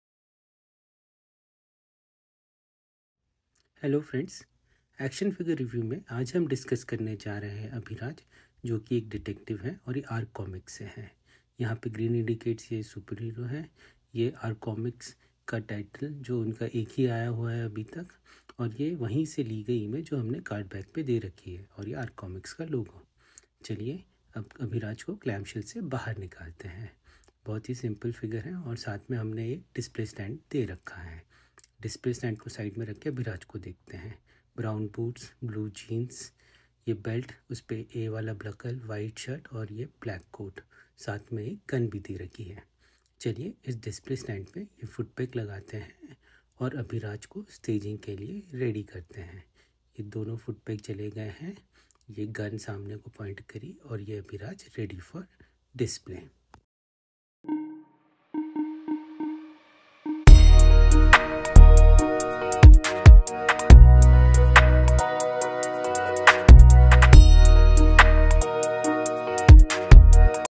Abhiraj | Arc Comics| Detective | Custom Action Figure | 1 Minute Brief
Presenting a 1 minute brief on the custom action figure of the flagship character of Arc Comics  -  “DETECTIVE ABHIRAJ" the one who loves solving mysterious  crimes. The figure is a minorly modified zombie lab Robin and the cardback is by yours truly. The figure + cardback is  fan made for his own personal collection and not intended for any commercial usage.